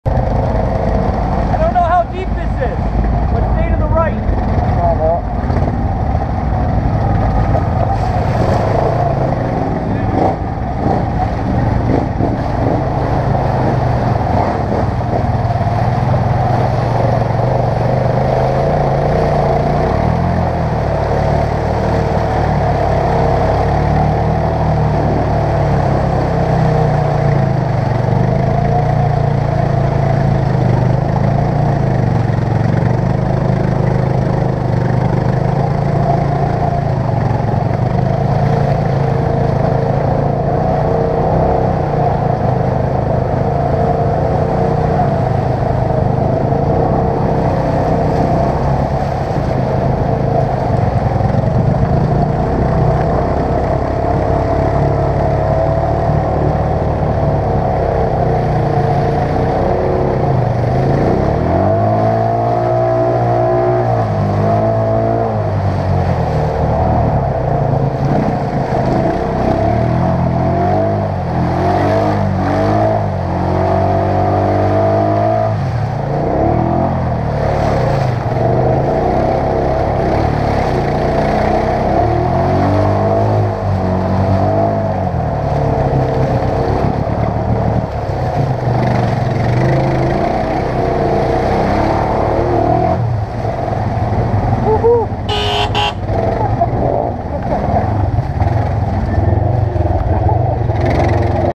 Honda Rubicon Mudding Clips #1
Made it through a decent mud hole. Then later the XMR 1000 just plows through it.

Recorded in Georgian Bay, Ontario, Canada